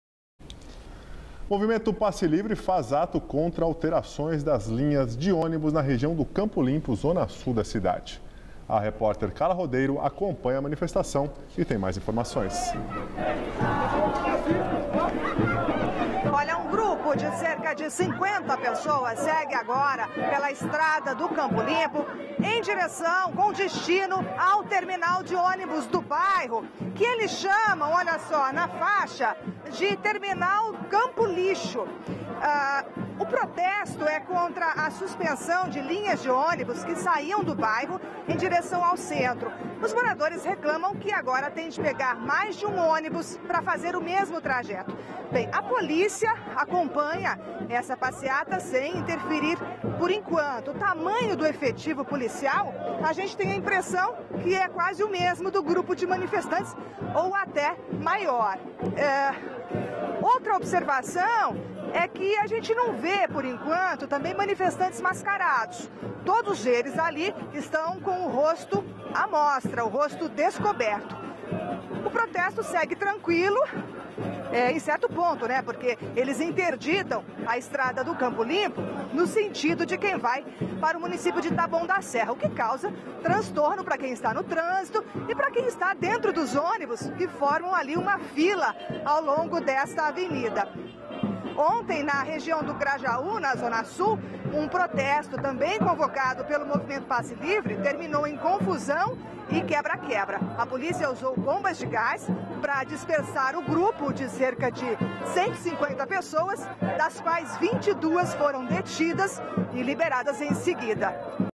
Protesto por ônibus no Campo Limpo
Movimento Passe Livre faz ato contra alterações das linhas de ônibus na região do Campo Limpo, zona sul da cidade. A repórter Carla Rodeiro acompanha a manifestação e tem mais informações.

Tema: Manifestação